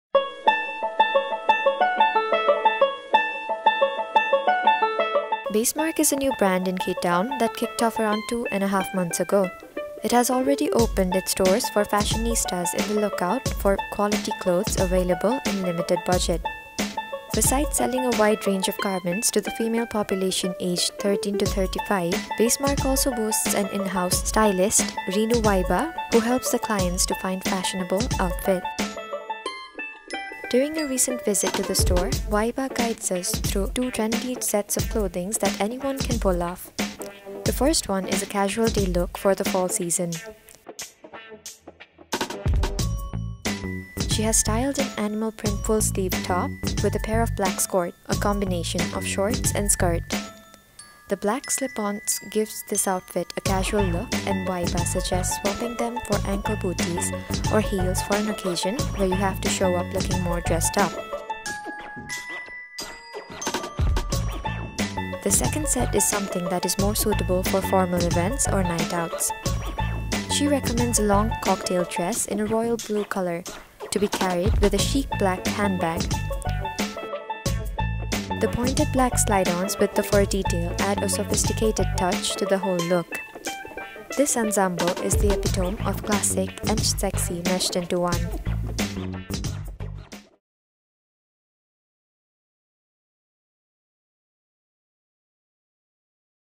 Basemark: A new brand in K town
Basemark is a new brand in K town that kicked off around 2 and a half months ago.  The brand targets fashionistas in the lookout for quality clothes available in limited budget.  Besides selling a wide range of garments to the female population aged 13 to 35, Basemark also boasts an in-house stylist, Reenu Waiba, who helps the clients to find fashionable outfits.

The first and only video content portal with entertainment, sports, politics and lifestyle content tailored for the Nepali audience.